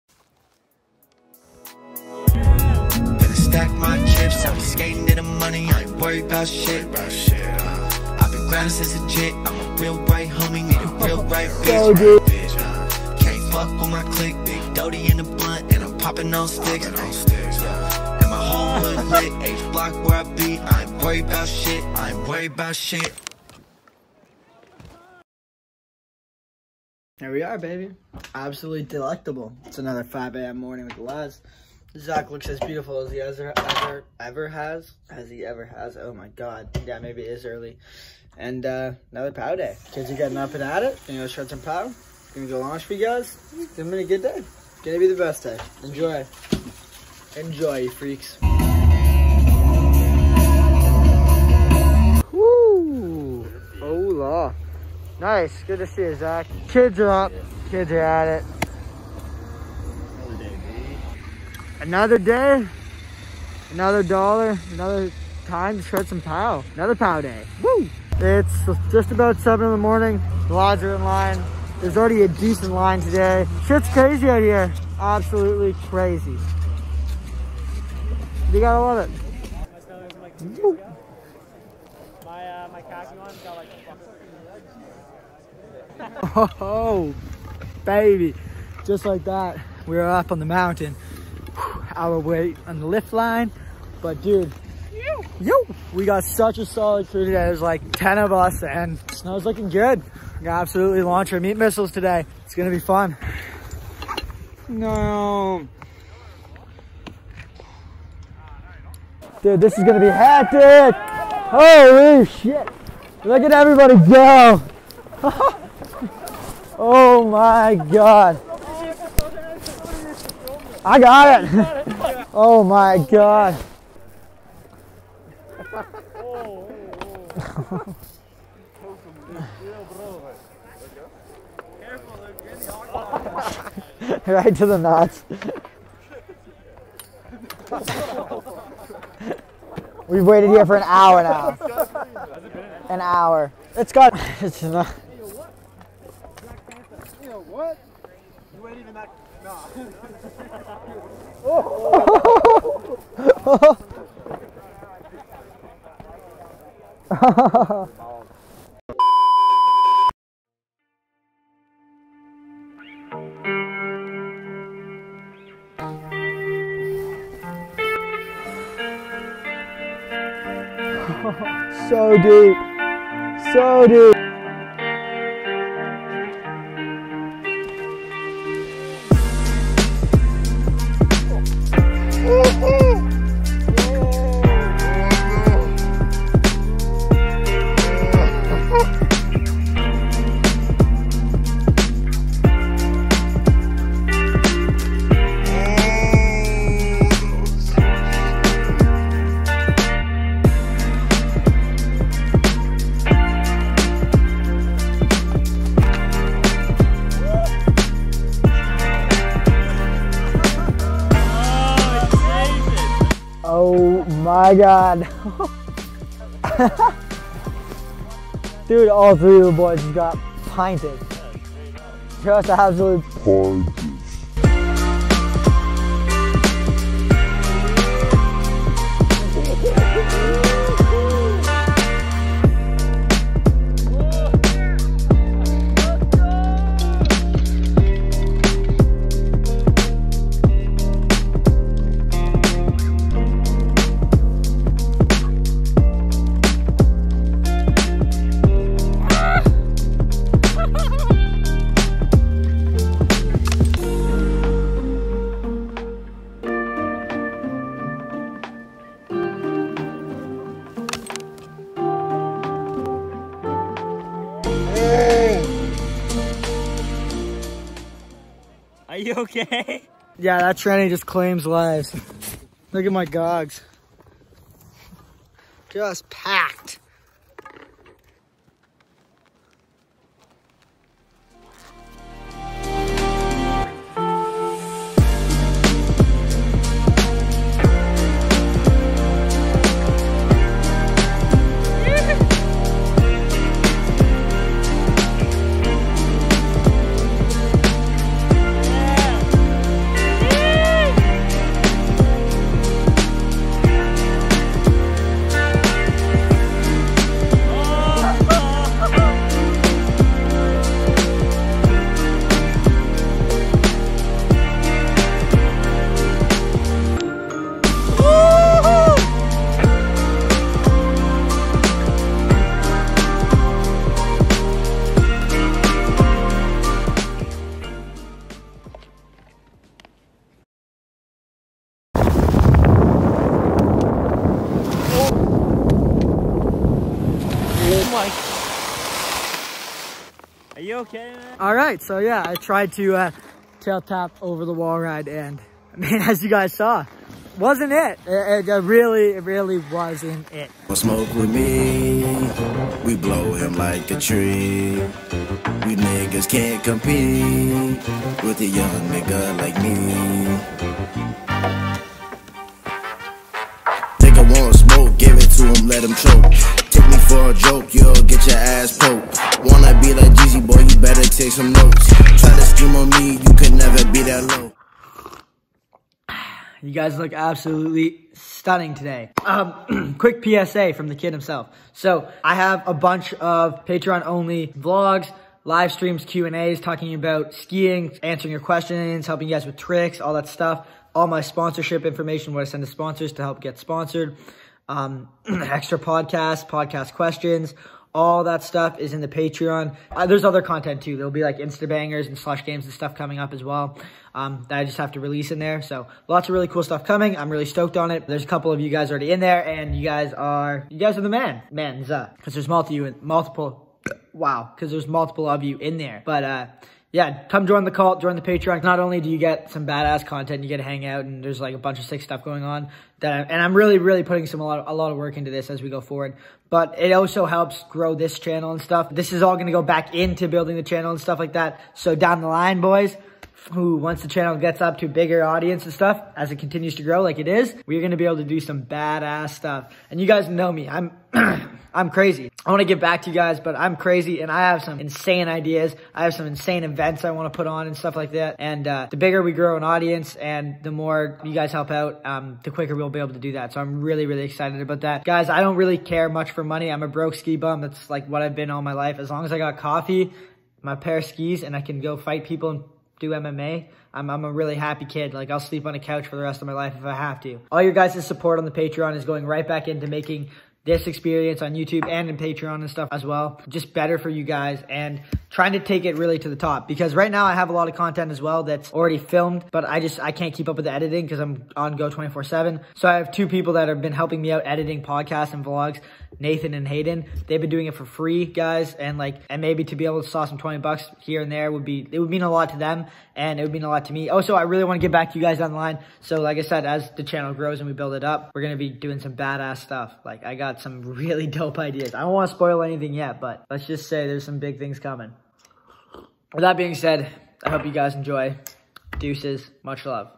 GNARLY TOMAHAWKS | DROPPING CLIFFS IN WHISTLER 🥵
👾 SLOPE ACADEMY ⤵️ (Best Freestyle Skiing Community & Coaching)

We got yet another deep pow day in Whistler and the lads sent it... maybe a little to hard 😂 Pretty unreal day filled with heavy slams and good times! Nick and Jake at it pretty decently on some of the nicer cliffs Whistler Backcomb has to offer 🛸  ENJOY 💕

SKIERS INCLUDE:
Zach Fillier, Nick Suchy, Jake Sandstrom, Lief Wilson, and Luka

EDITED BY: Nathan Benson